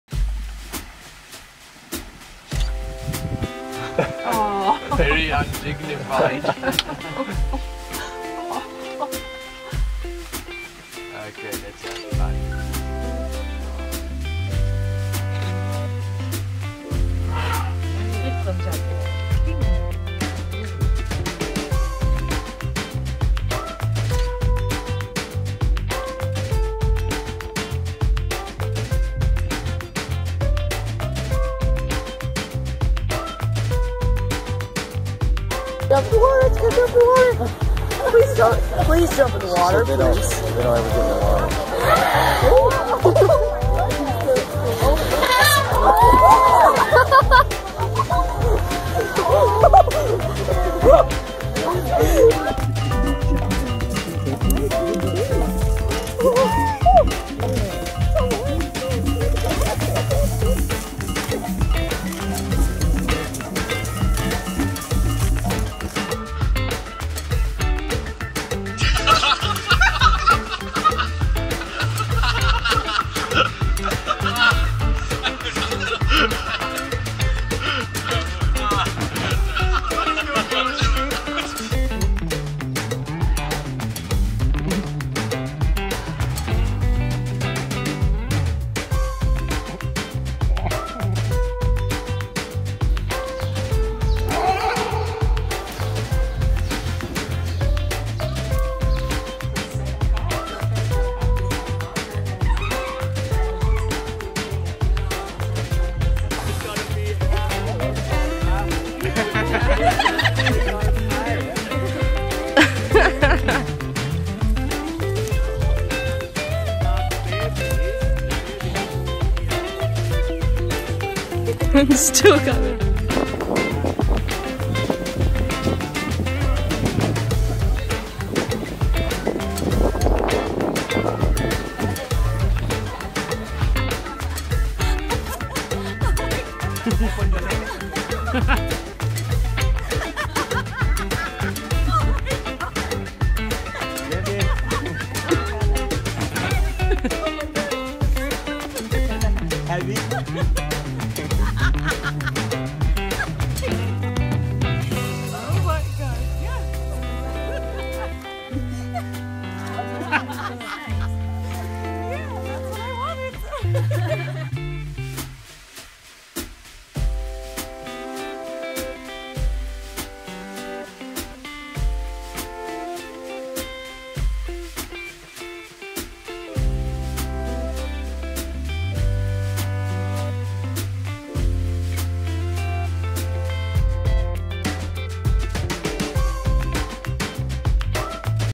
Funny Elephants Compilation
You will witness the cutest, funniest elephants and baby elephants in this compilation. These larger than life wild animals are too amazing and silly to miss.